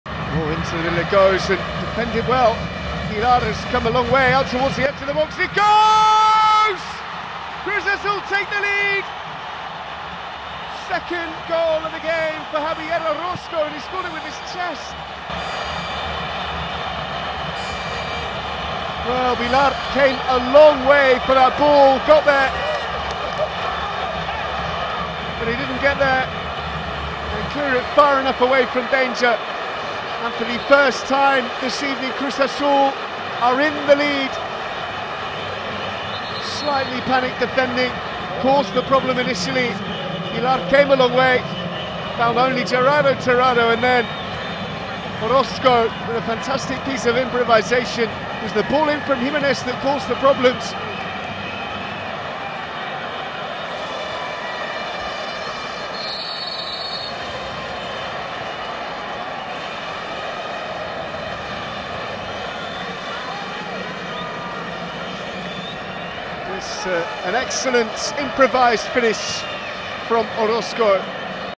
Chest the best! Clever deflection for goal in Mexico!
A good punch out by the keeper, but in comes the volley and Javier Orozco's cleverly chested deflection puts Cruz Azul 3-2 up away at Morelia. The match (in Mexico) finished 3-3.